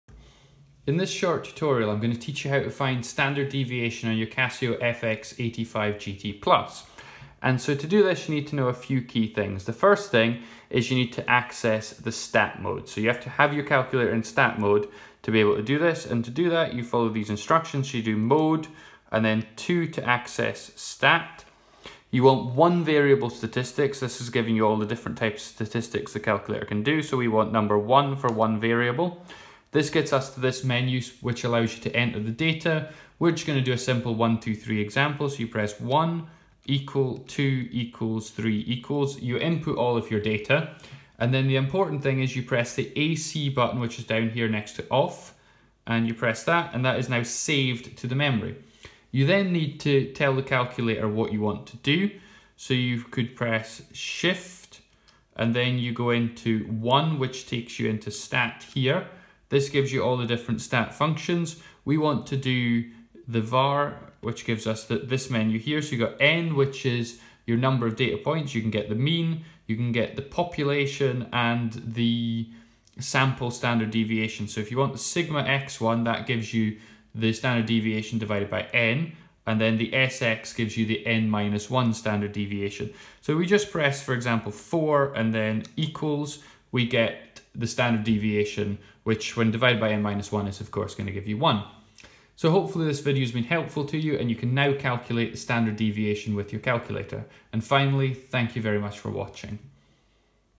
Casio FX-85GT Plus Standard Deviation and Mean
In this short tutorial I will teach you how you can do basic statistics with the Casio FX-85GT Plus. I will show you how to calculate the population and sample standard deviation, enter and save data and then find the mean.